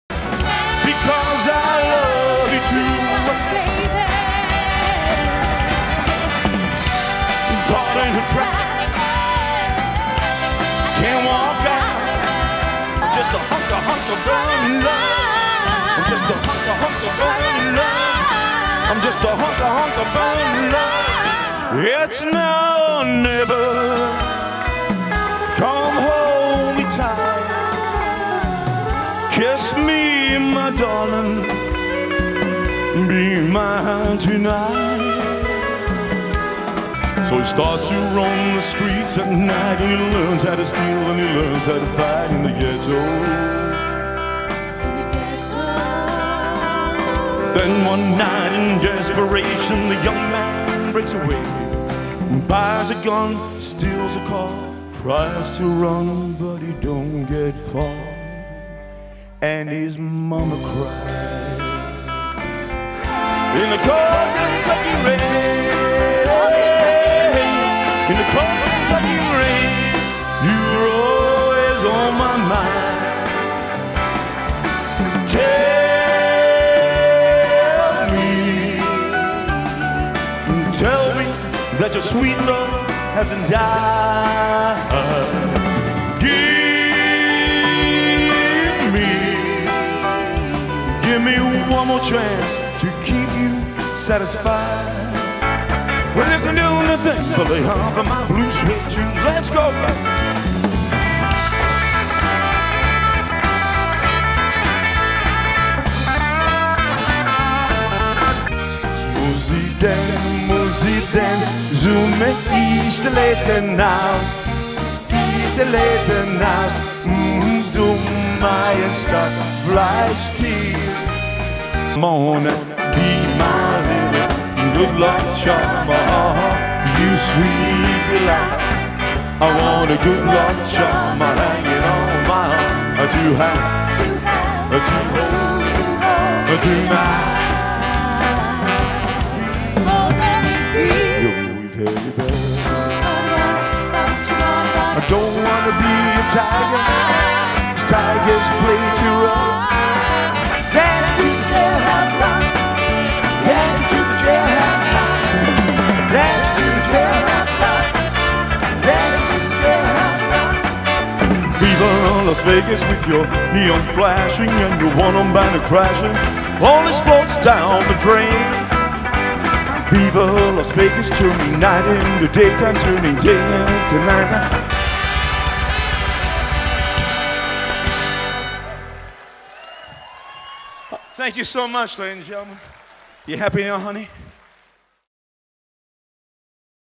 Australian Elvis Impersonator Peter Triantis
Elvis is alive and well, well you would have thought so after seeing this show in Hobart at the Wrest Point Casino May 2023.